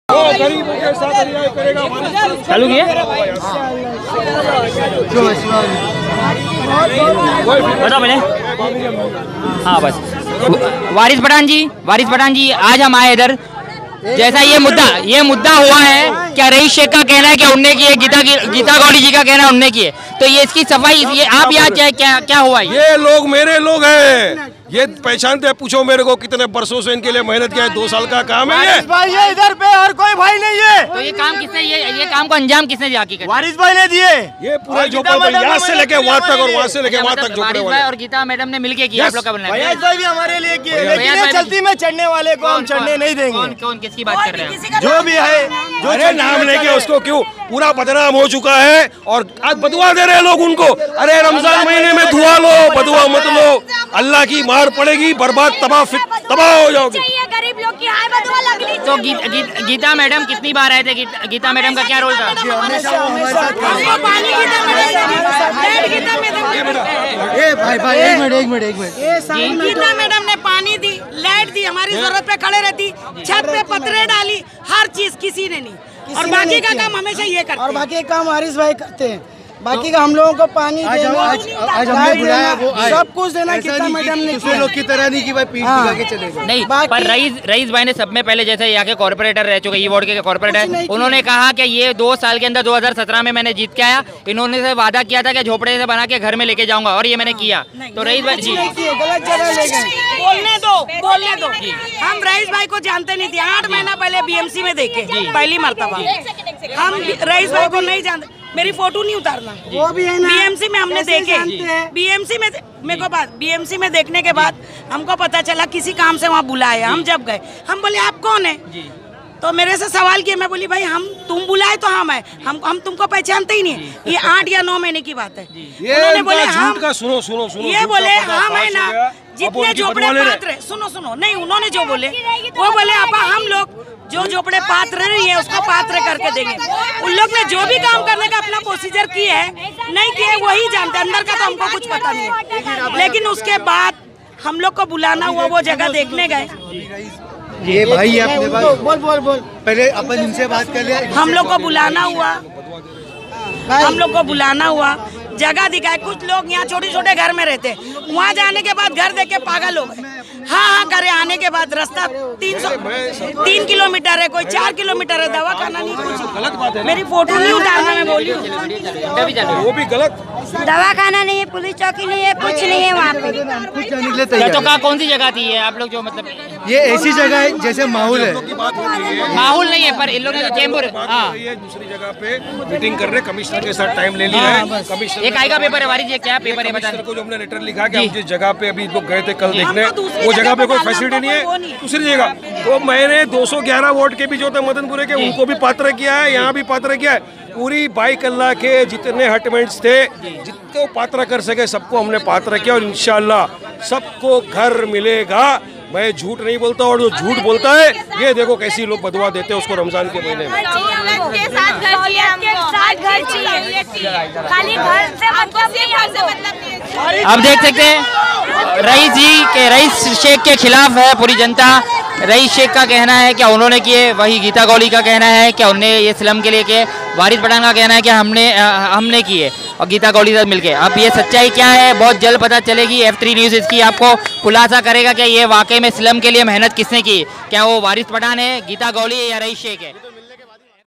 MLA Waris Pathan Ne Kaha Ramadan Mahine Mein Dua Lo Baadwa Mat Lo | Development | Byculla | Mumbai
☝🗣 MLA Waris Pathan, Corporator Geeta Gawli VS Corporator Rais Shaikh (Mumbai)

🔥 MLA Waris Pathan Mile Slum Walo Ko Aur Byculla Ki Rajneeti Mein Lagi Aag

🎥 Watch Exclusives Interview & Spot Video Koun Hain Slum Walo Ke Liye Maseeha 👇👇

☝ Corporator Rais Shaikh Ne Kaha Yeh Unki Mehnat Hai Ke Slum Walo Ko Ghar Milega

🗣 Wahi Corporator Geeta Gawli Ne Kaha Yeh Kaam Unki Kayi Saal Ki Mehnat Ka Anjaam Hai

❗ Kal MLA Waris Pathan Bhi Pouche Slum Walo Se Milne Aur Kaha Yeh Kaam Geeta Gawli Aur Mein Waris Pathan Ne Kiya Hai

😳 Byculla Mein Yeh Aab Aam Ho Gaya Hai MLA Waris Pathan, Corporator Geeta Gawli To Kabhi Corporator Rais Shaikh Ek Kaam Hota Hai Aur Dawa Sub Karte Hai

🤔 Sach To Slum Wale Hi Jaante Hai Ke Akhir Yeh Mehnat Kiski Hai Aur Koun Sacha Hai Aane Wala Election Teh Karega